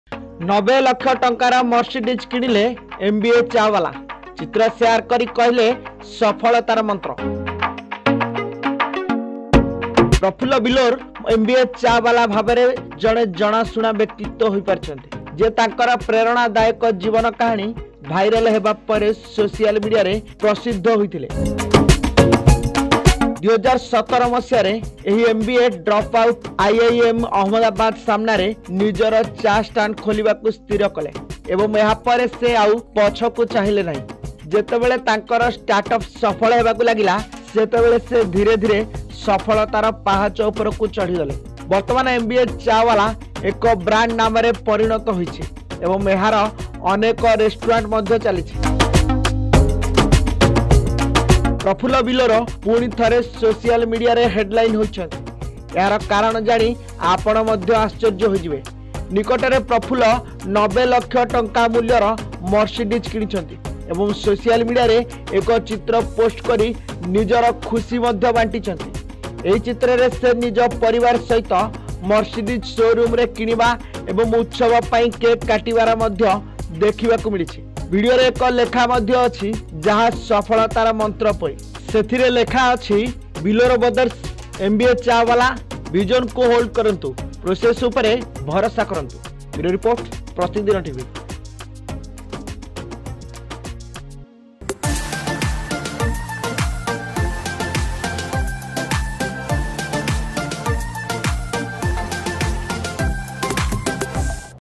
୯୦ ଲକ୍ଷ ଟଙ୍କାର ମର୍ସିଡିଜ୍ କିଣିଲେ ଏମବିଏ ଚା ୱାଲା ।I PratidinTv
୯୦ ଲକ୍ଷ ଟଙ୍କାର ମର୍ସିଡିଜ୍ କିଣିଲେ ଏମବିଏ ଚା ୱାଲା ।I